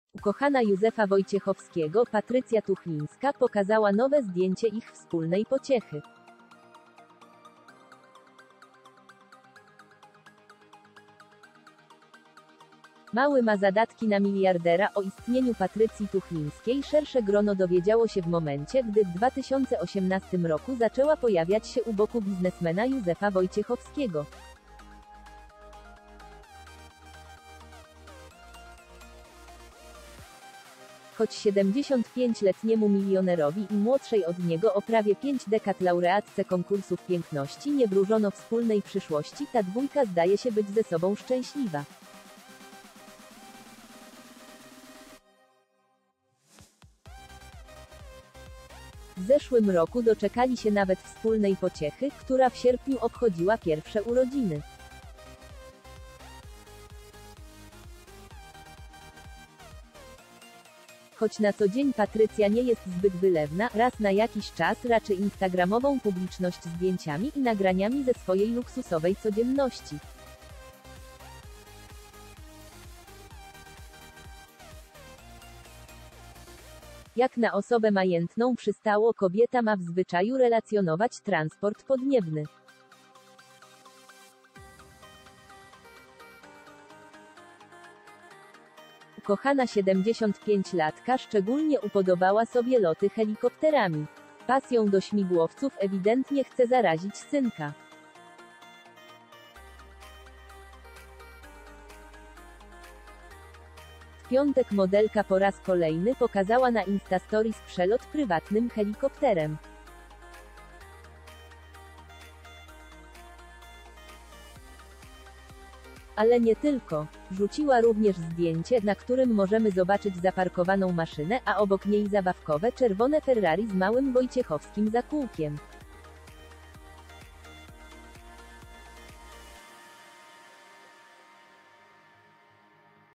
Roczny syn 75 letniego Józefa Wojciechowskiego i Patrycji Tuchlińskiej pozuje w dziecięcej wersji fe
Roczny syn 75-letniego Józefa Wojciechowskiego i Patrycji Tuchlińskiej pozuje w dziecięcej wersji ferrari na tle helikoptera
Ukochana Józefa Wojciechowskiego, Patrycja Tuchlińska, pokazała nowe zdjęcie ich wspólnej pociechy. Mały ma zadatki na miliardera?